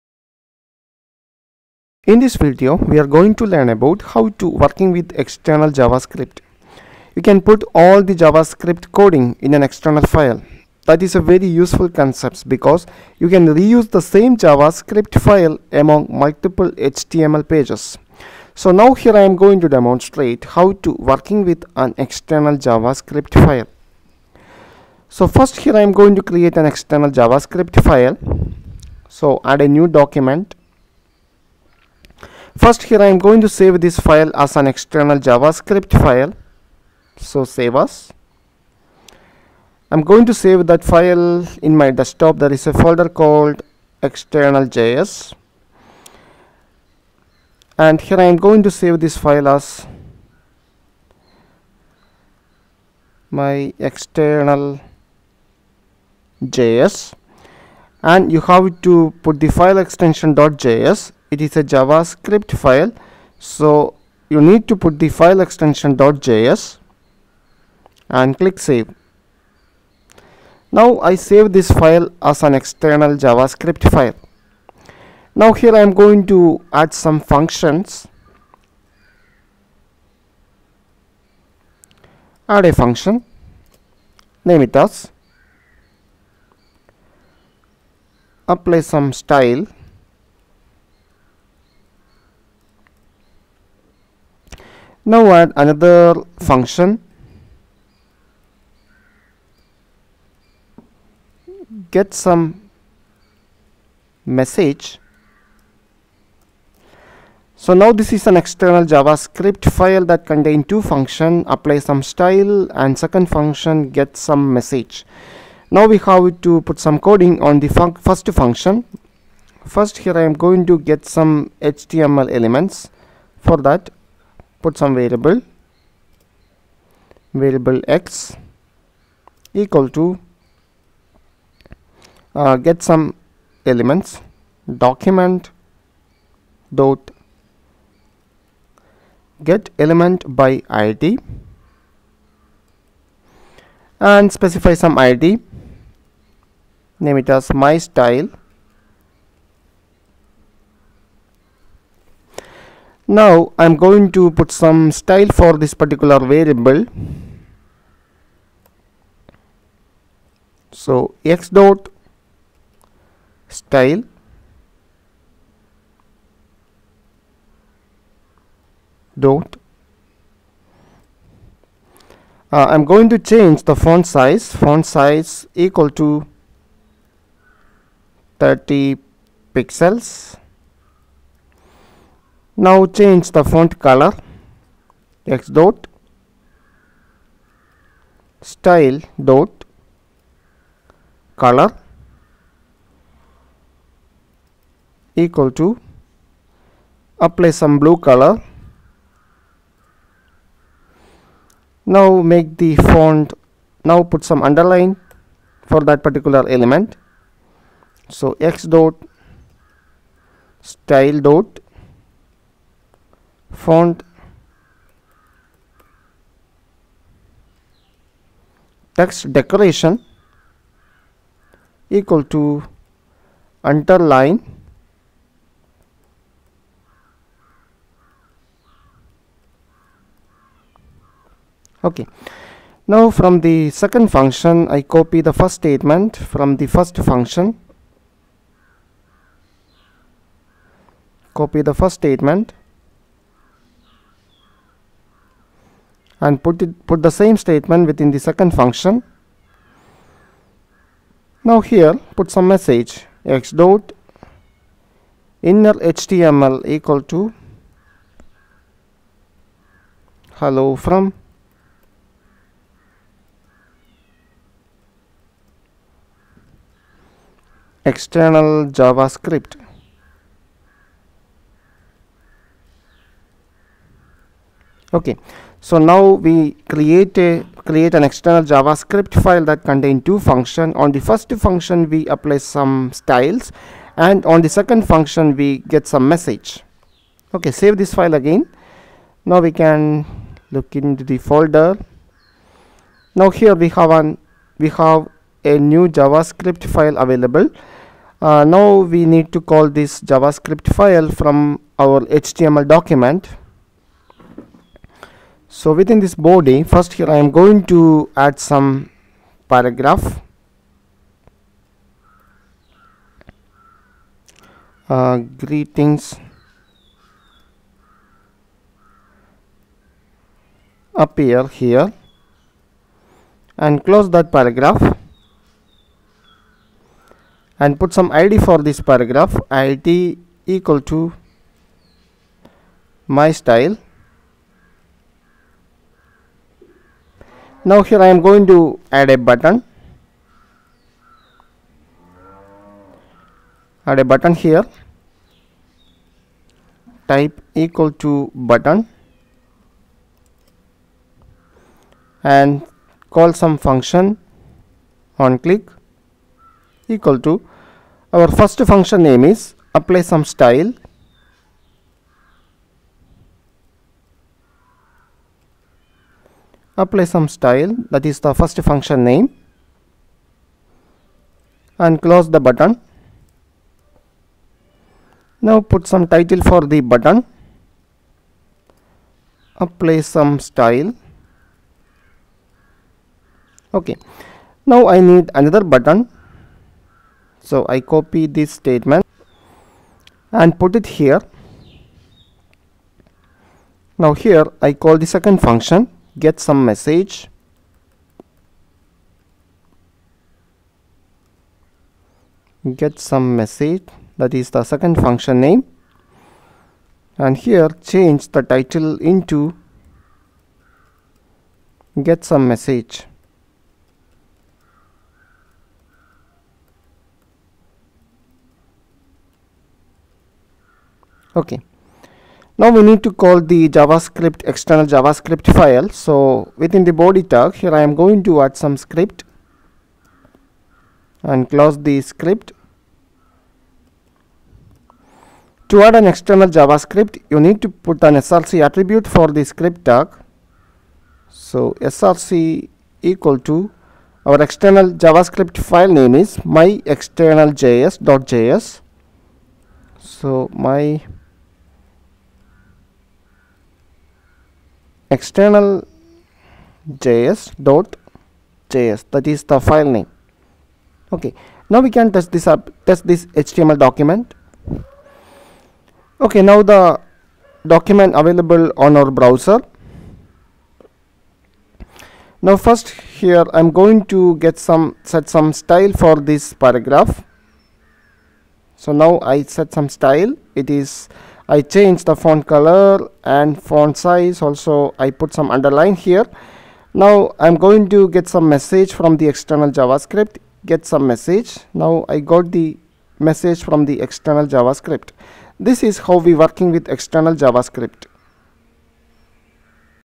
JavaScript - 06 - External JavaScript
How to working with external JavaScript in an html page. You can reuse the same external JavaScript file among multiple html pages.